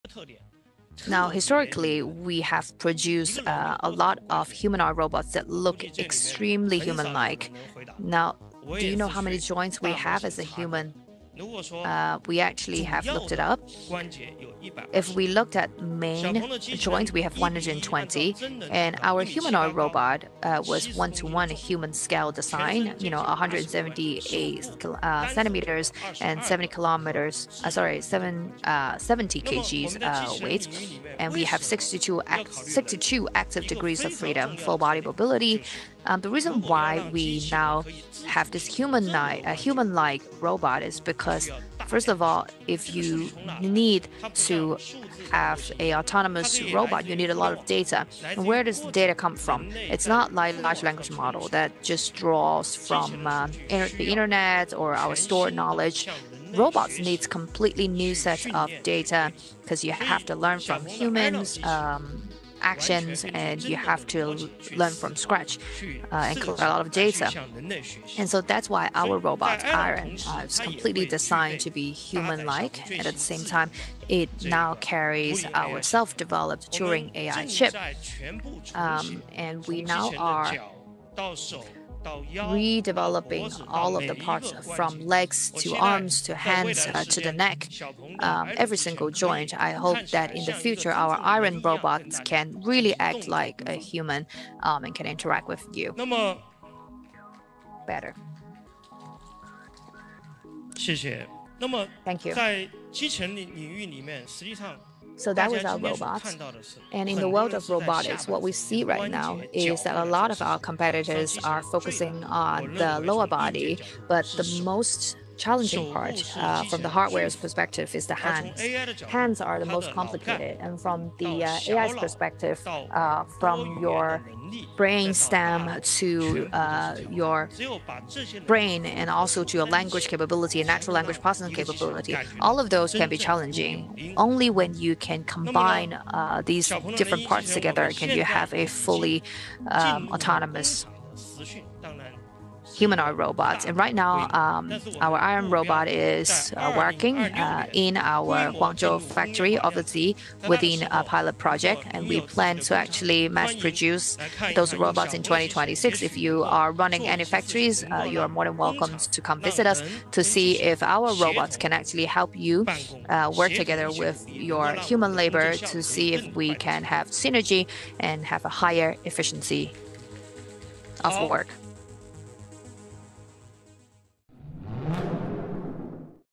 Xpeng’s Iron Robot vs Tesla’s Optimus: Who Wins the AI Race?
The race for humanoid robots is heating up, and Xpeng just raised the bar with its new creation: Iron. Standing at 178 centimeters and packed with artificial intelligence muscle, this machine could change how humans work—forever.

Xpeng’s Iron will feature 62 active degrees of freedom and run on a custom-built Turing artificial intelligence chip capable of processing 30 billion parameters. Backed by an investment of over 5 billion British pounds, this project is part of a larger tech push that includes flying cars, autonomous driving, and more.

In this video, we take a closer look at what makes Iron unique, what Xpeng’s goals are, and how it stacks up against competitors like Tesla’s Optimus. Will this robot really enter mass production next year? Can it collaborate with humans? And what does this mean for the future of work?

Watch until the end to find out what He Xiaopeng thinks the future looks like, and whether Iron can truly become a part of everyday life.